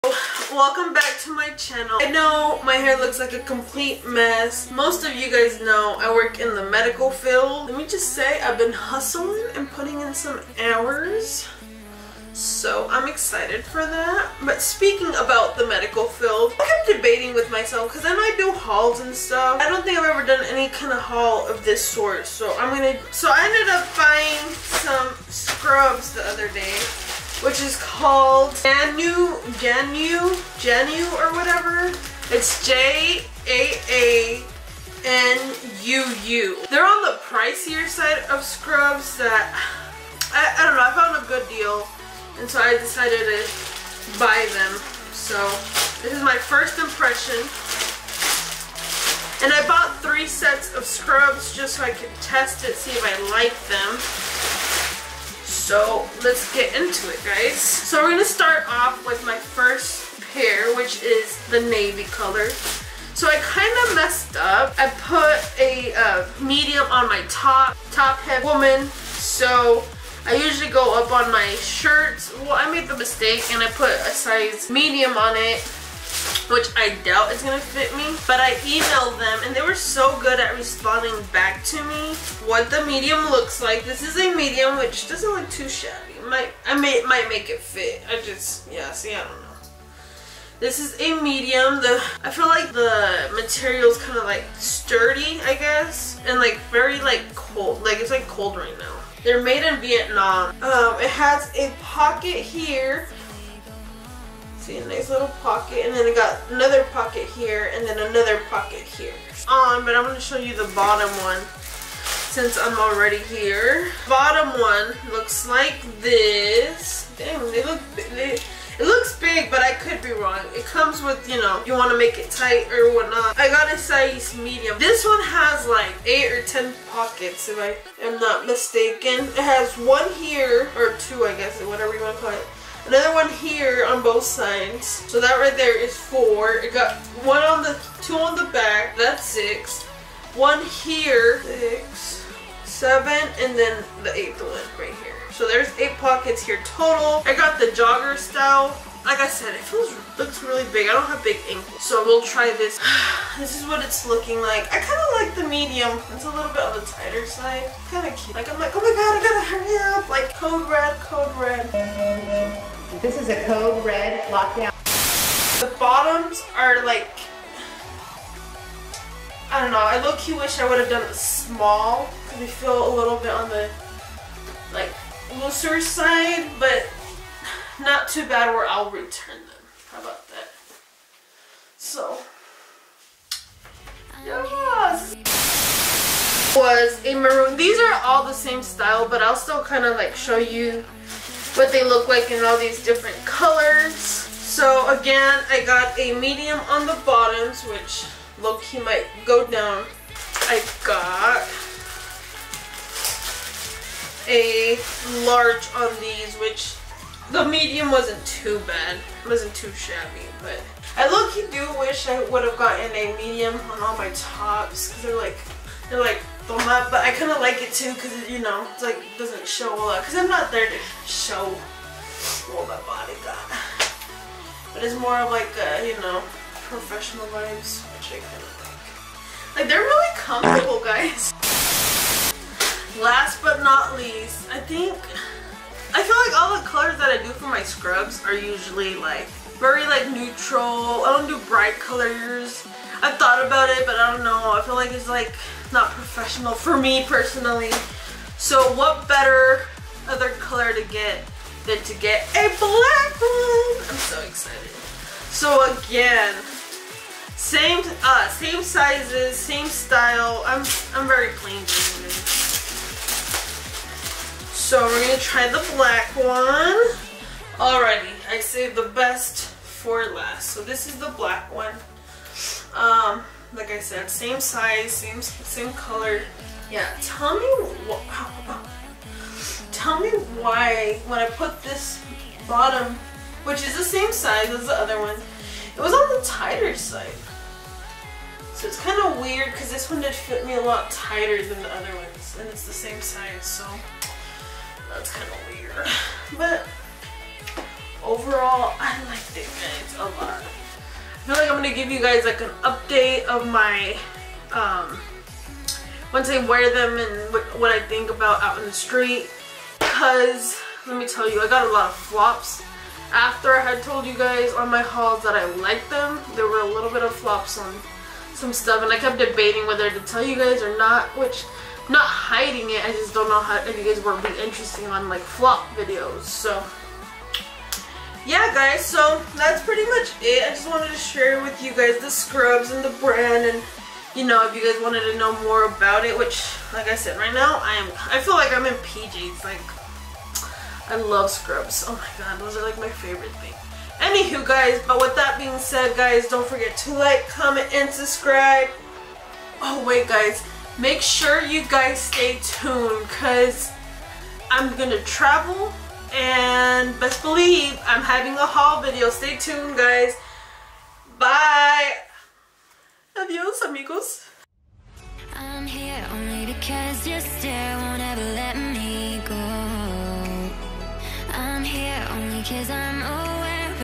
JAANUU SCRUBS | Try On Haul | Top Heavy Woman
Hope This Video Is Very Helpful! And If You Decide To Buy Them They DO CATCH ALOT OF LINT!!